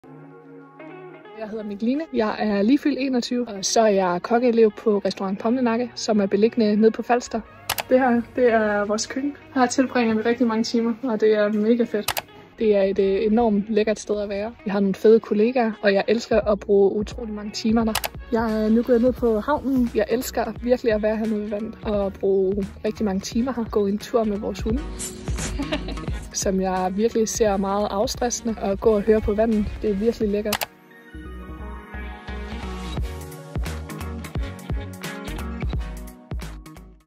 Mikkeline Møller Rasmussen
Mød Mikkeline – en sprudlende kokkeelev fra Falster, der elsker at lave mad og drømmer om at åbne sin egen restaurant.